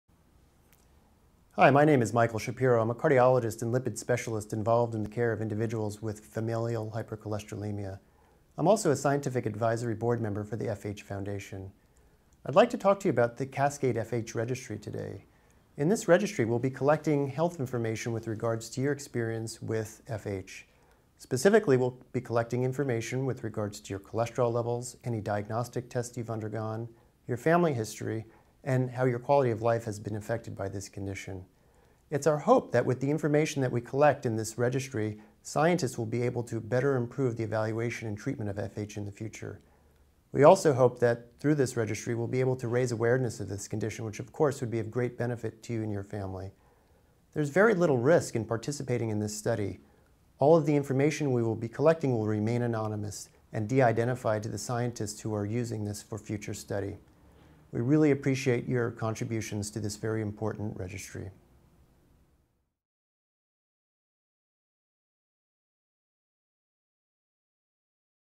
Michael Shapiro, DO speaker about the CASCADE FH® Registry
Familial Hypercholesterolemla (FH) is one of the most common life-threatening family disorders. If you have FH, at least someone else in your family also has FH. Individuals with FH remain vastly under-diagnosed and under-treated and are at an increased risk for heart disease and heart attacks at a very young age. However, FH is manageable with the right care.

The goal of the CASCADE FH® is to collect information on individuals with FH and their family members. The information is stored in a highly secure portal located on the FH Foundation website. Your information will be shared with our research partners in a de-identified manner.